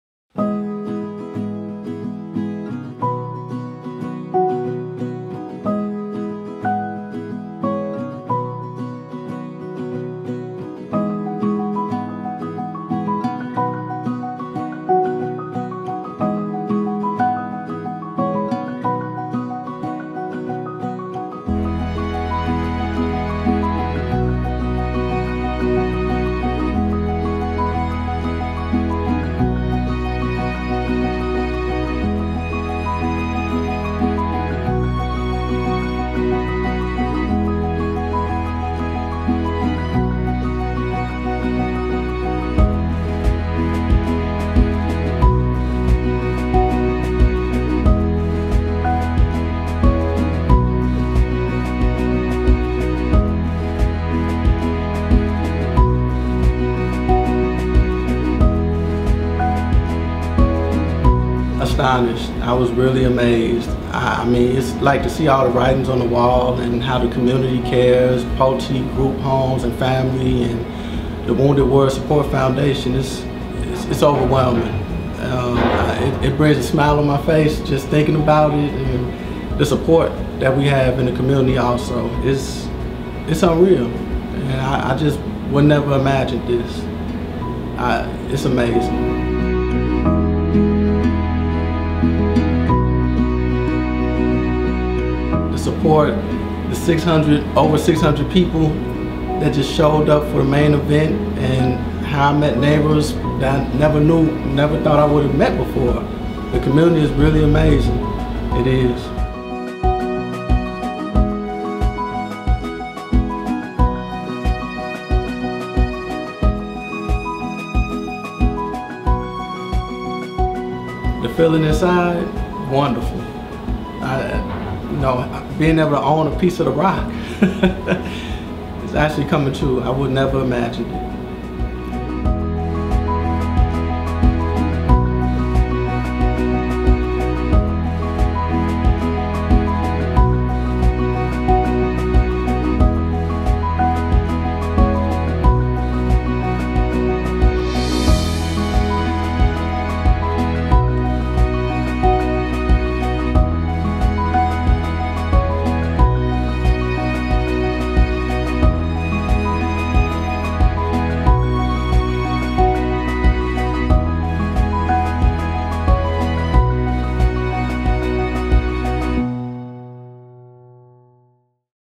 The Richmond Family Sees Their Home Envelope with Notes of Love
A native of Chicago, Retired Sgt. Richmond joined the U.S. Army in early 2006 and served two tours in Operation Iraq Freedom and Operation Enduring Freedom with the 4th Brigade Combat HHC/Mortars-Scouts.

Sgt. Richmond was surprised with the announcement that he and his family will be receiving a new home. The Illinois Division of PulteGroup with support from its trade partners and vendors, will build, decorate and donate the new home and will be completely mortgage-free.

PulteGroup's Built to Honor™ program recognizes and thanks returning military personnel who have been injured during their term of service by providing mortgage-free homes to veterans and their families.